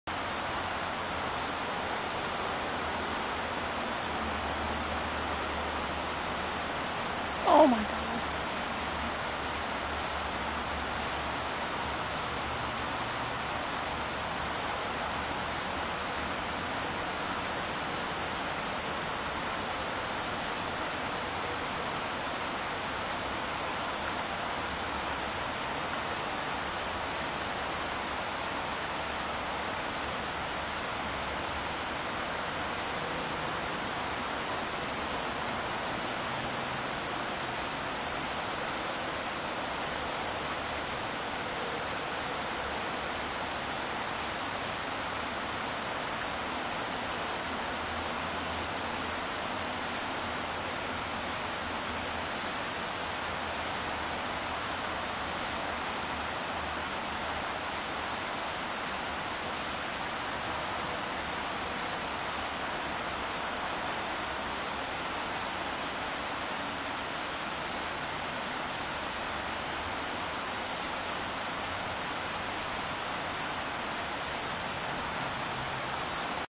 Juvenile Great Blue Heron tries & fails to eat dead King Salmon
Tumwater Falls Park, WA, Sept. 2017